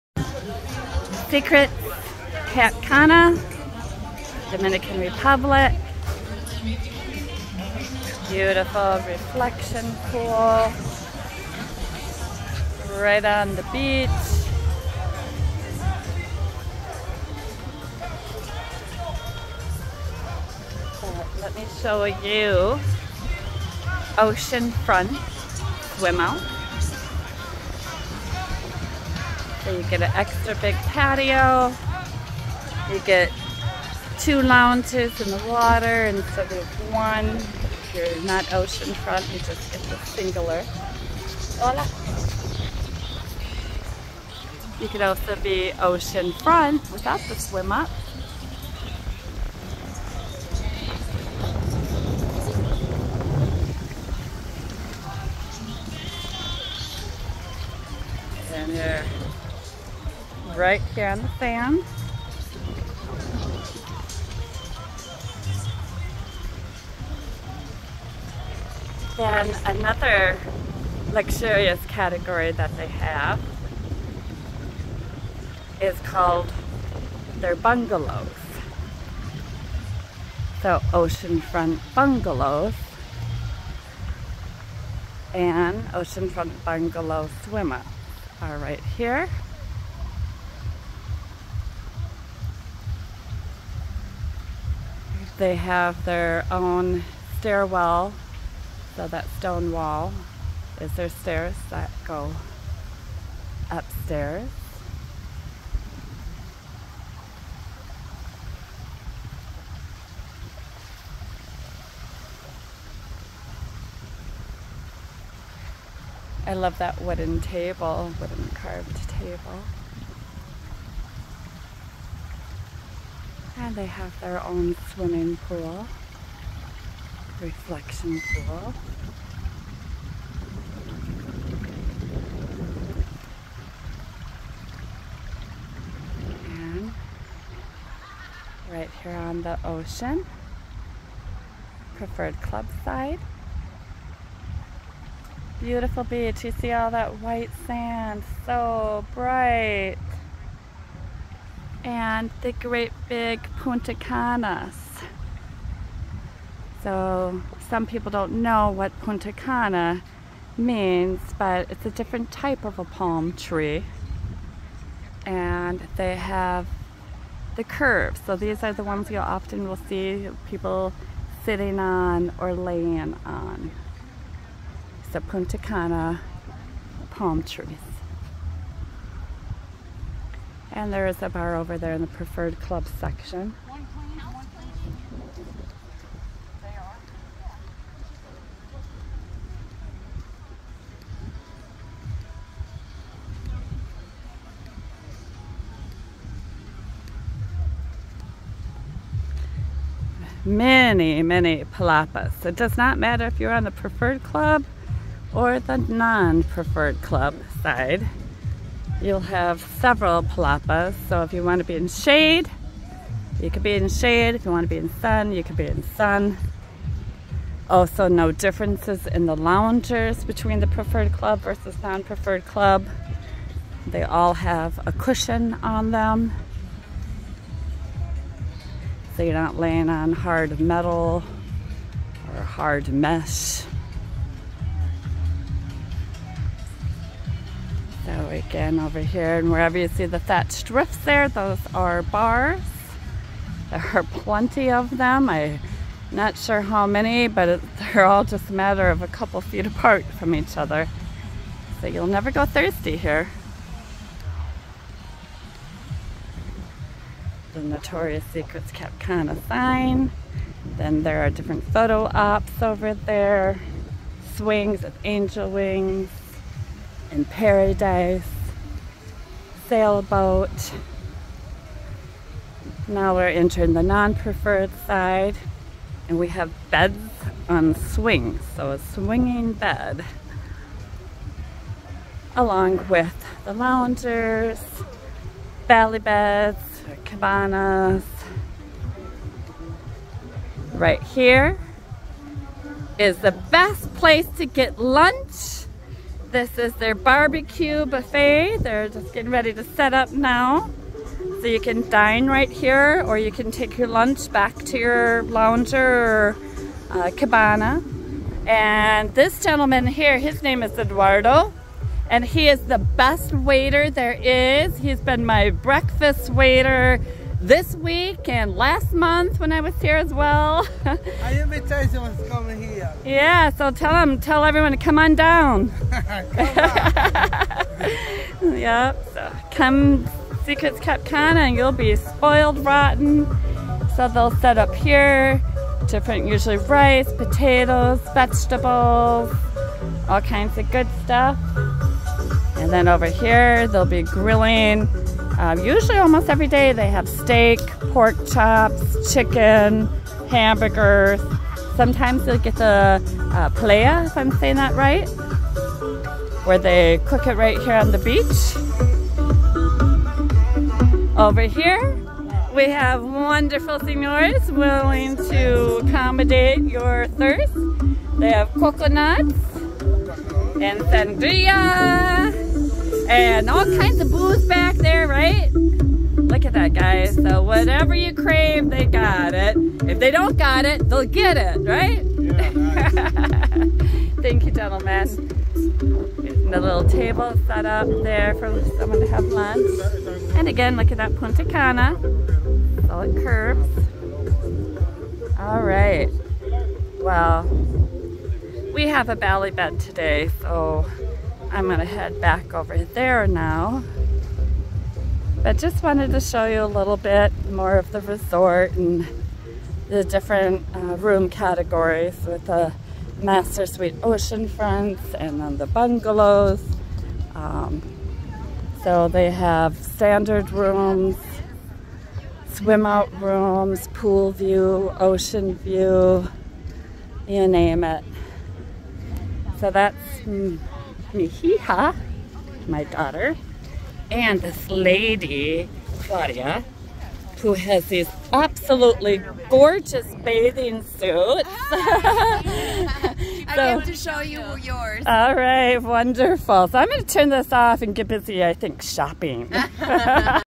Secrets Cap Cana Beach Overview
Here you will get a peek at the Beach BBQ, Main Pool, Exterior of Preferred Club Ocean Front Room Categories & both sides of the Beach .